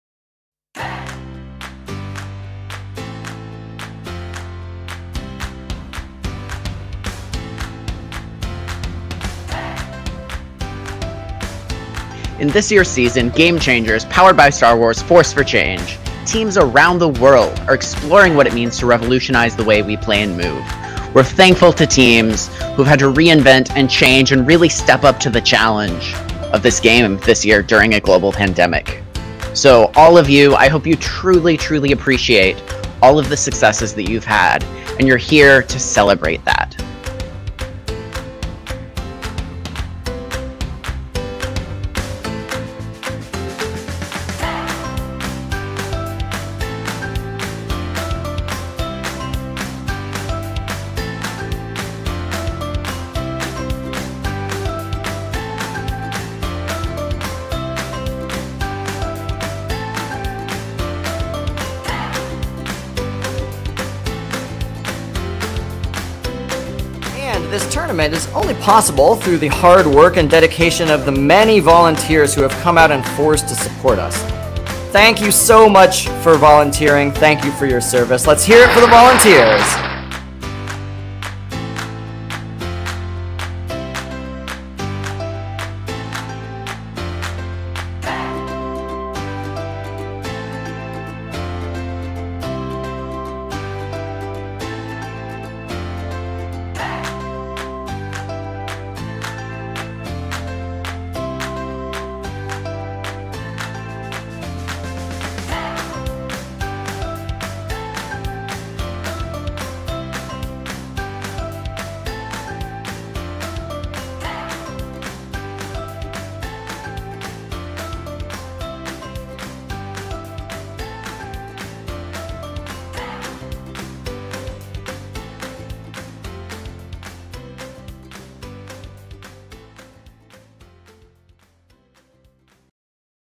NorCal FLL Challenge RePLAY Season Remote Qualifier Tournaments
A look into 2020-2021 RePLAY Season Qualifier Tournaments organized by Playing At Learning.